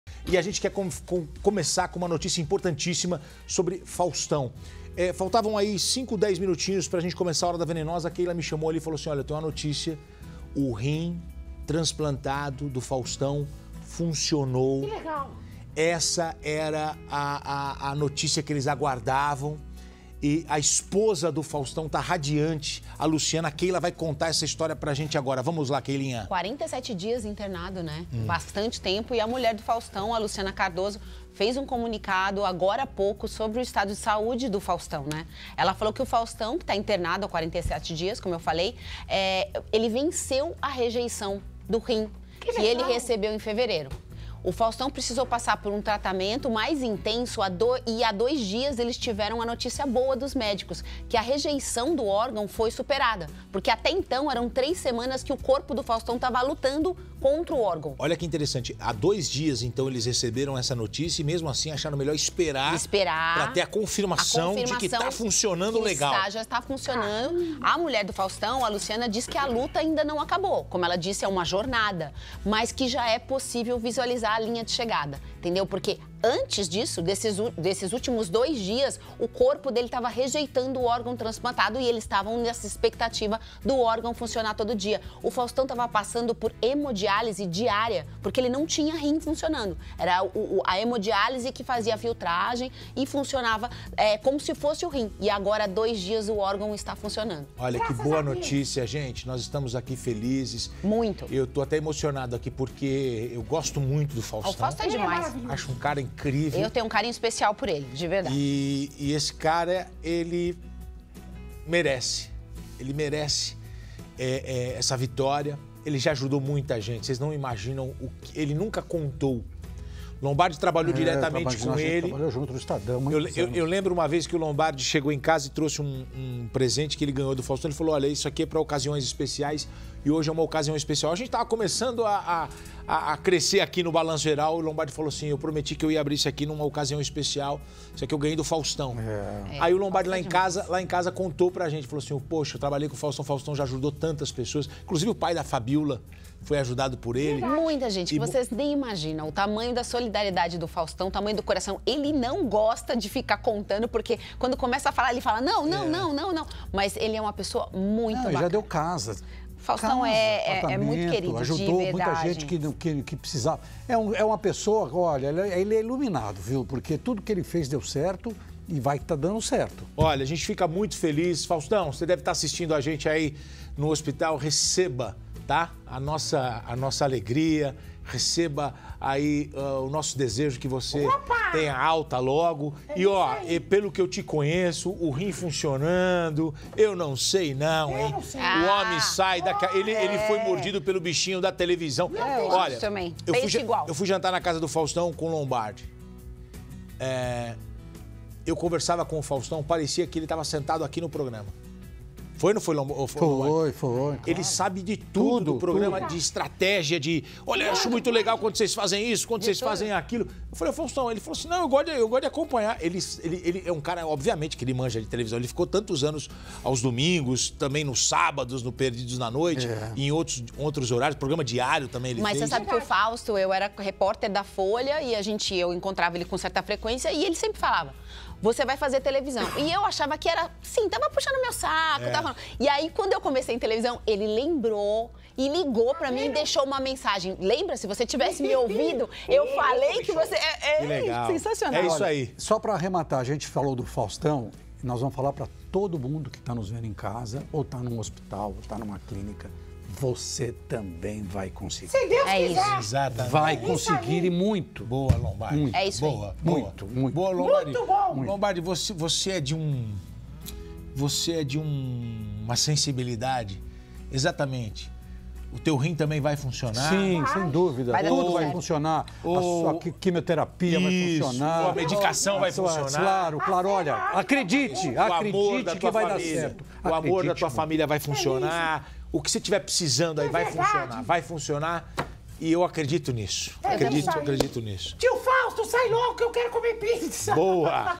Faustão está com o rim funcionando após transplante, informa esposa
Luciana Cardoso, esposa de Faustão, fez um comunicado sobre o estado de saúde do marido dela. Ela falou que ele está internado há 47 dias e que “venceu” a rejeição do rim que recebeu em fevereiro. Faustão precisou passar por um tratamento mais intenso e há dois dias eles tiveram a confirmação de que a rejeição foi superada. O apresentador fez o transplante de rim no dia 26 de fevereiro. Em agosto de 2023, ele já havia se submetido a outro transplante, o de coração.